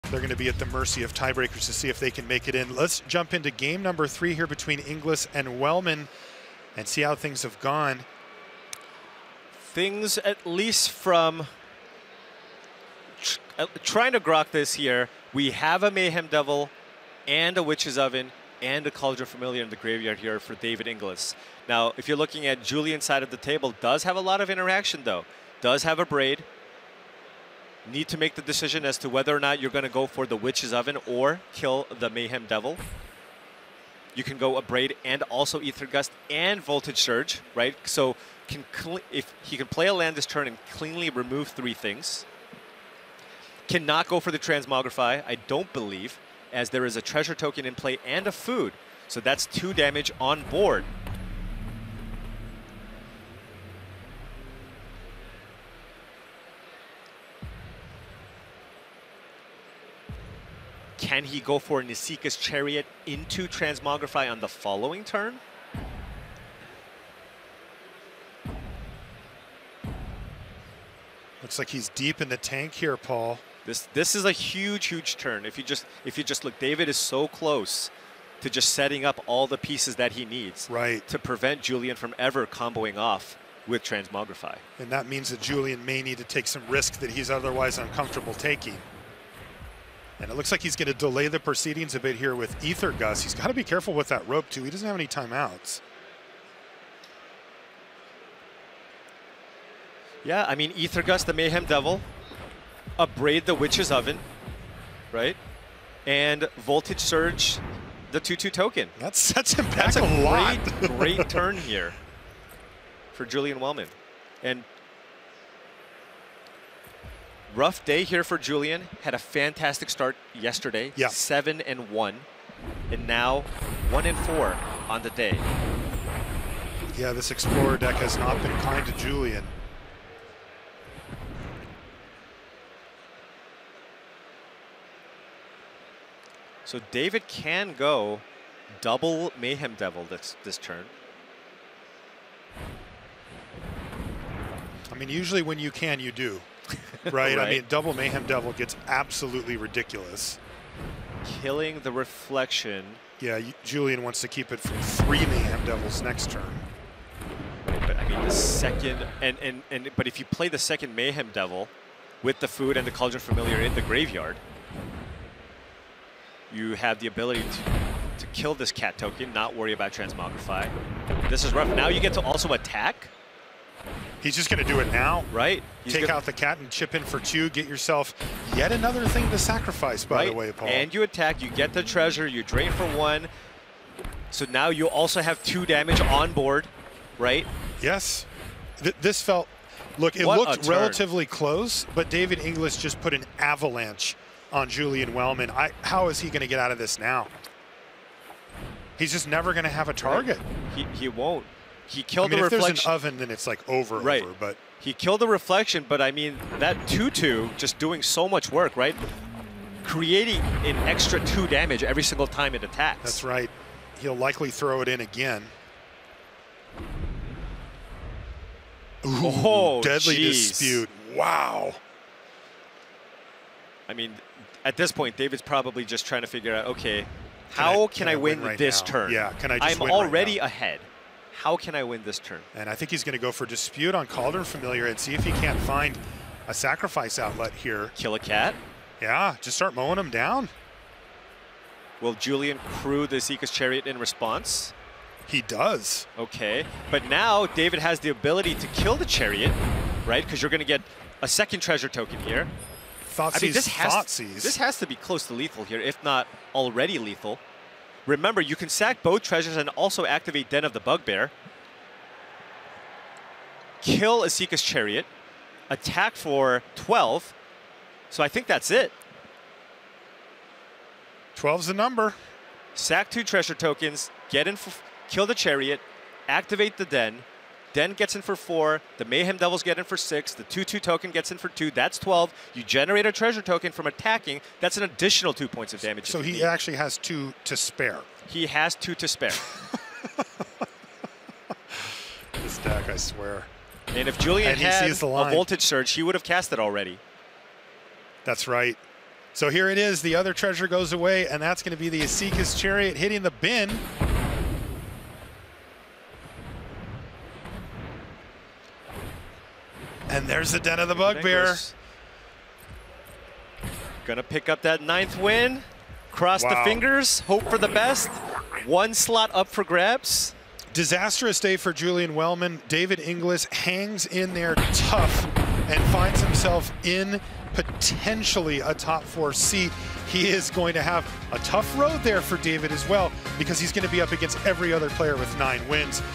Julian Wellman vs David Inglis | Round 14 | Magic World Championship XXVIII
Julian Wellman (Temur Transmogrify) vs. David Inglis (Rakdos Sacrifice)

Find full coverage, decklists, and more from Magic World Championship XXVIII